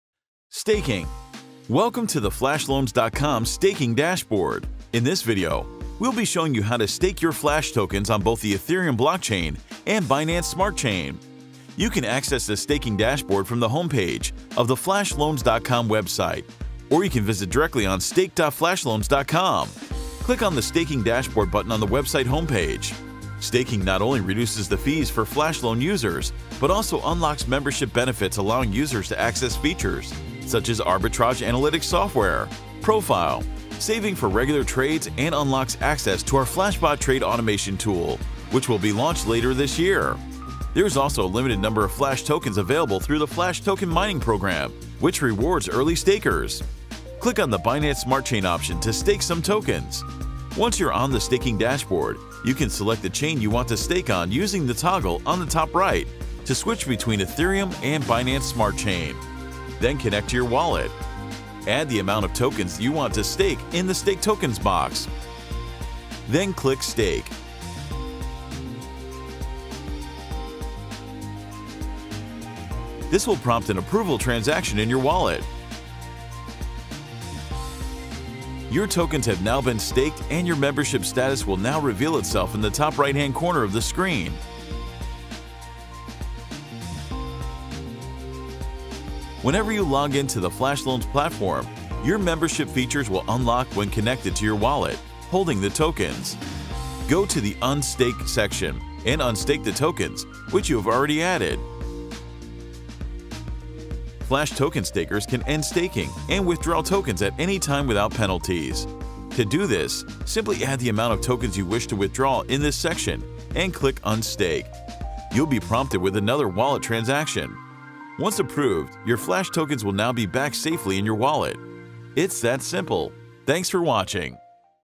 FLASHLOANS - STAKING
A full tutorial on how to stake FLASH tokens and activate membership benefits